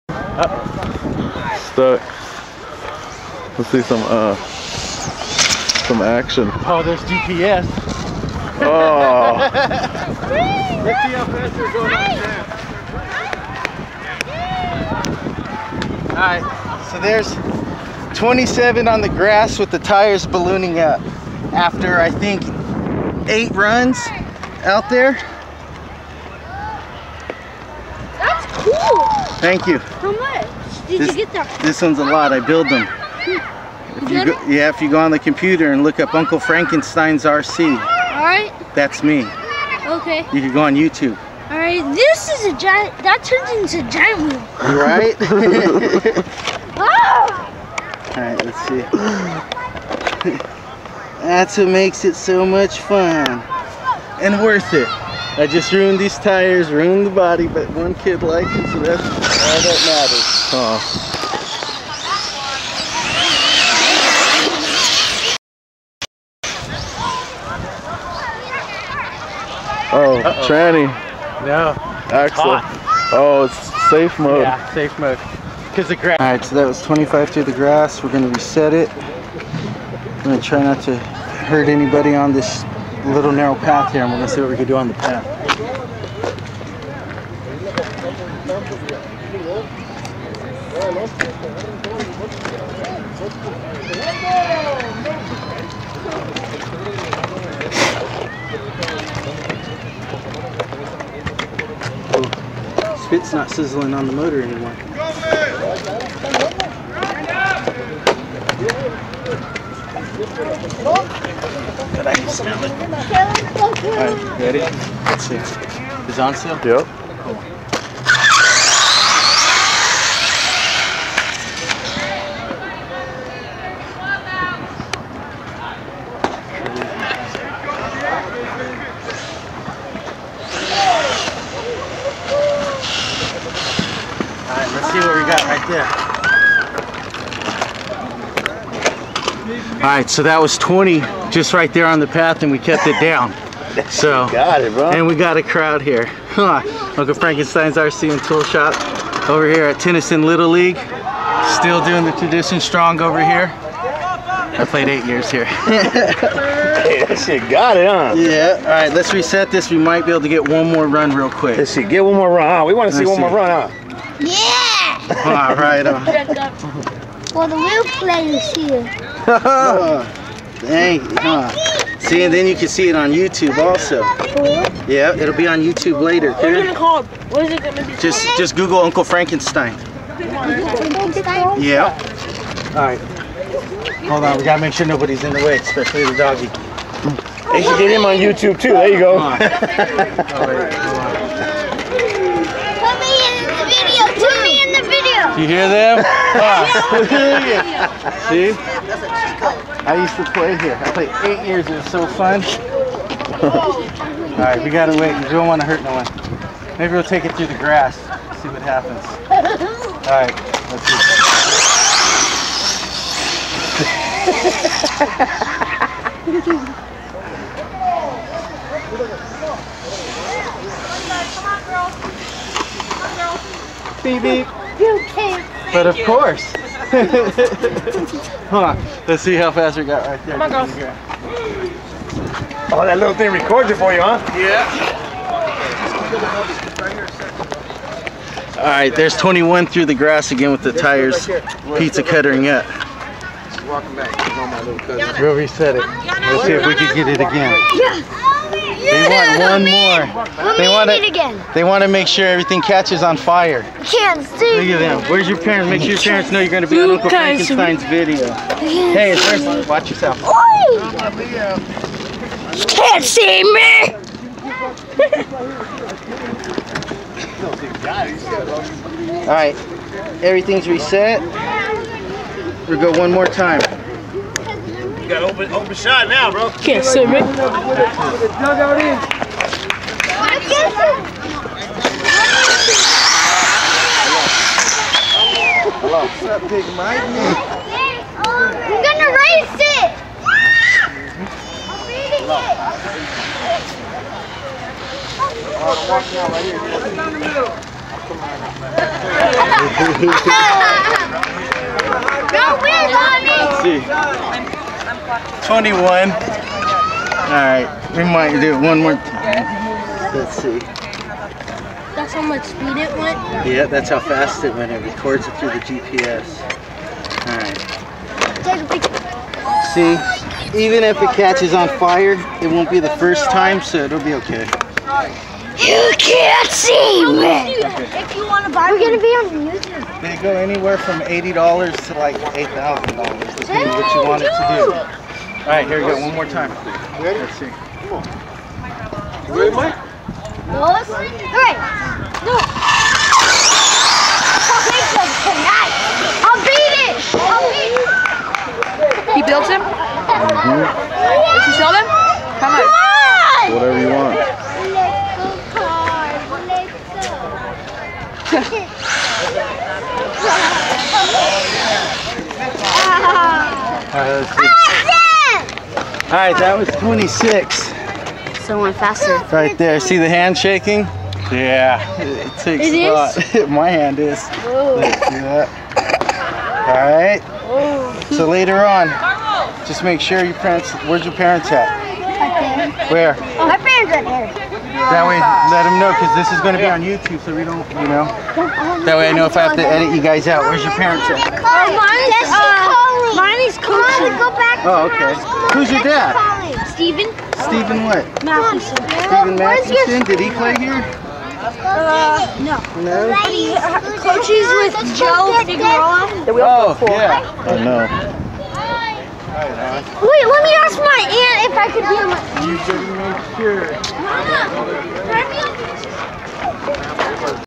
KYOSHO INFERNO SPORT BRUSHLESS CONVERSION BY UNCLE FRANKENSTEIN'S RC
PLAYING AROUND ON DIRT TIRES TRYING NOT TO KILL THE BUGGY. putting a quick show for some kids and some adults at my old baseball field . TENNYSON AMERICAN LITTLE LEAGUE.  A GREAT TIME IN LIFE BASEBALL WAS ;O)